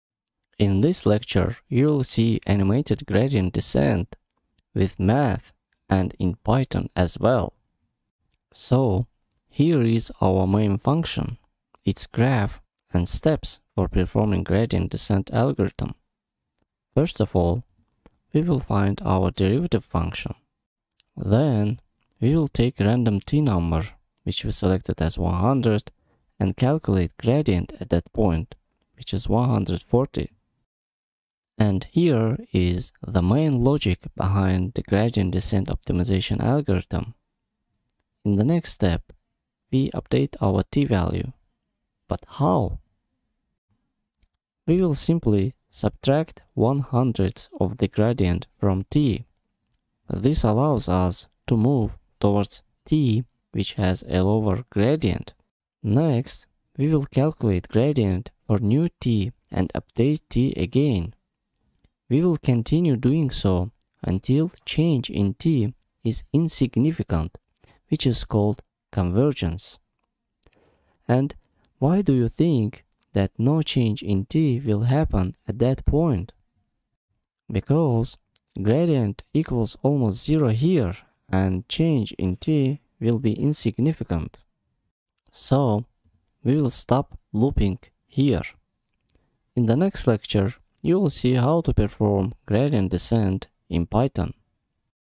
Lecture 7 - Animated Gradient Descent - Deep Learning and Neural Networks
Lecture 7
In this lecture you will see animated Gradient Descent with math and in python as well.

So, here is our main function, its graph and steps for performing gradient descent Algorithm. First of all, we will find our derivative function. Then we will take random T number, which we selected as 100 & Calculate gradient at that point, which is 140.

And here is the main logic behind the gradient descent optimization algorithm. In the next step we update our T value. But how? We will simply subtract 1 hundredth of the gradient from T. This allows us to move towards T which has a lower gradient.

Next we will calculate Gradient for new T and update T again. We will continue doing so until change in T is insignificant, which is called convergence. Why do you think that no change in T will happen at that point? Because Gradient equals almost zero here and change in T will be insignificant.
So, we will stop looping here. In the next lecture you will see how to perform gradient descent in python.